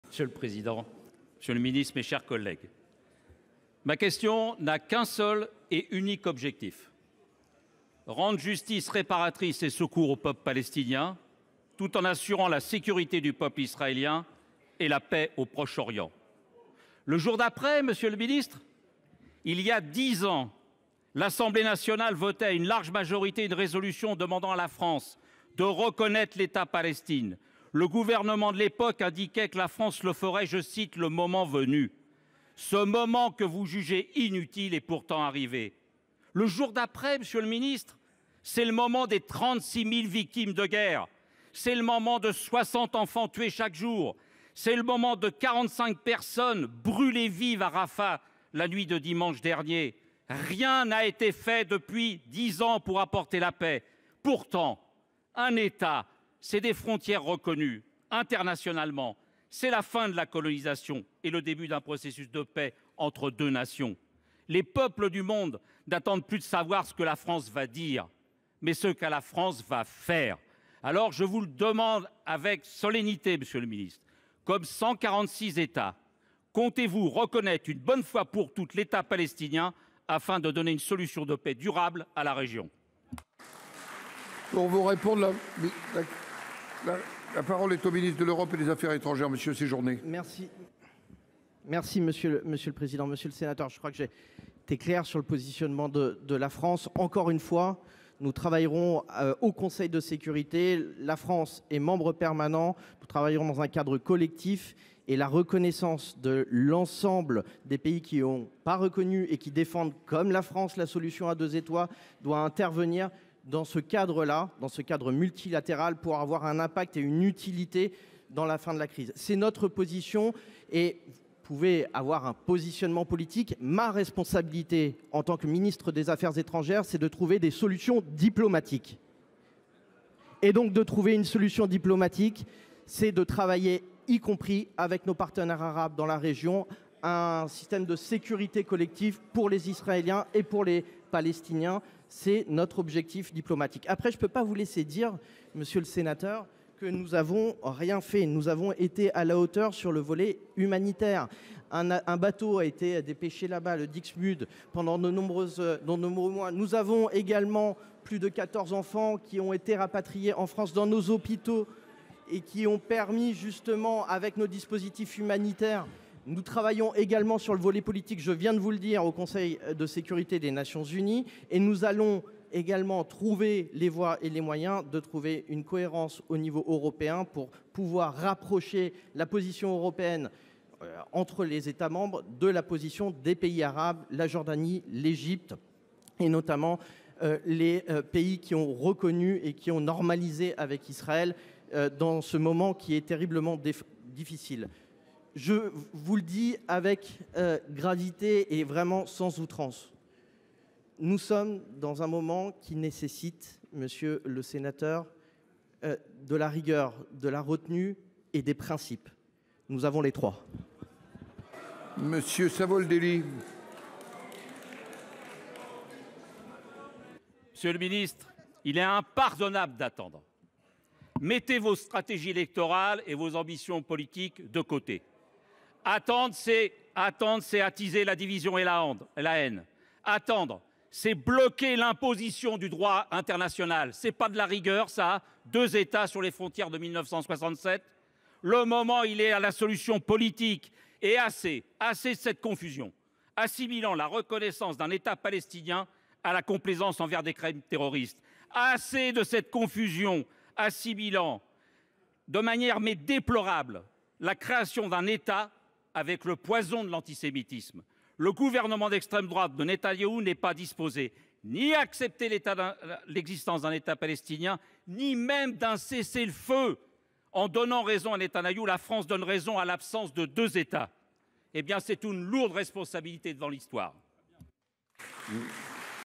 Coup de colère pour la Palestine !
➡️ "36 000 victimes de guerre. 60 enfants tués par jour. 45 personnes brûlées vives à Rafah. Rien n'a été fait depuis 10 ans pour apporter la paix ! Que fait la France !?" Pascal Savoldelli, Sénateur PCF